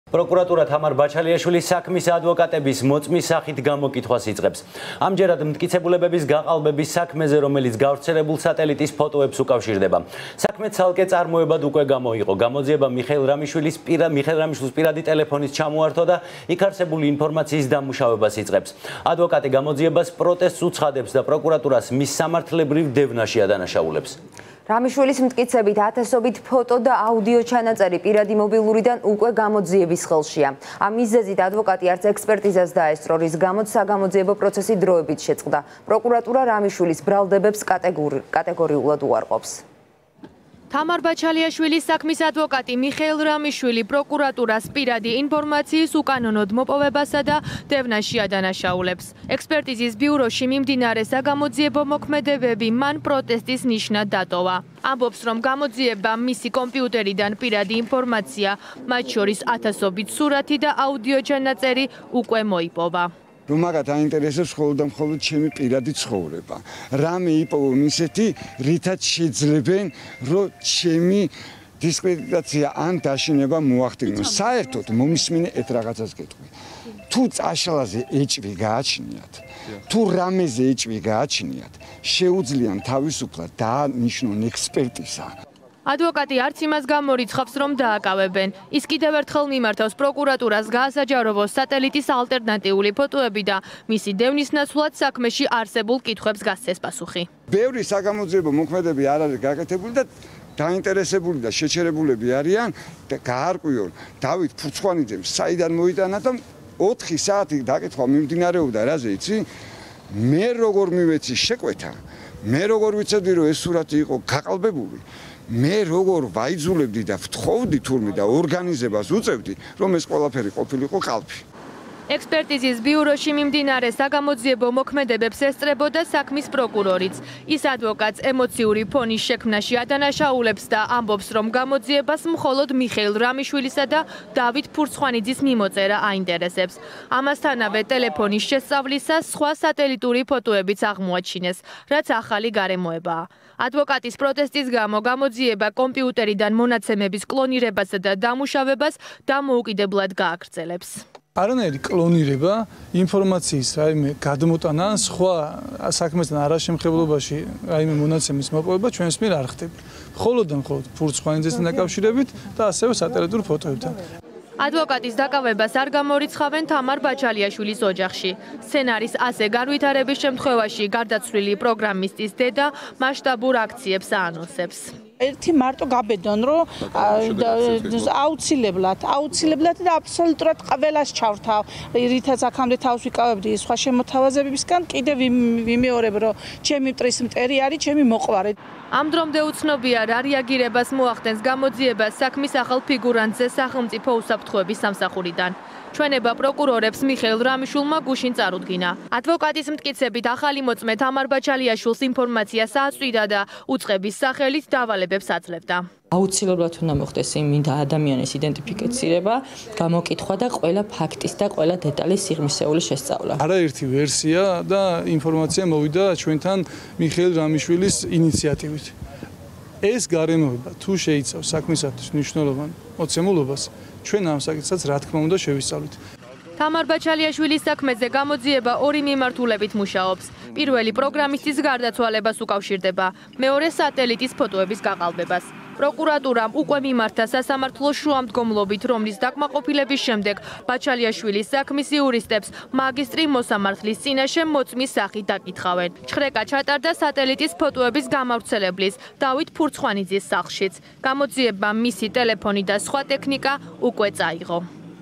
რამიშვილი პროკურატურას პირადი ინფორმაციის უკანონოდ მოპოვებასა და დევნაში ადანაშაულებს
▪️ პროკურატურა ბაჩალიაშვილის საქმის ადვოკატების მოწმის სახით გამოკითხვას იწყებს. ამჯერად, მტკიცებულებების გაყალბების საქმეზე, რომელიც გავრცელებულ სატელიტის ფოტოებს უკავშირდება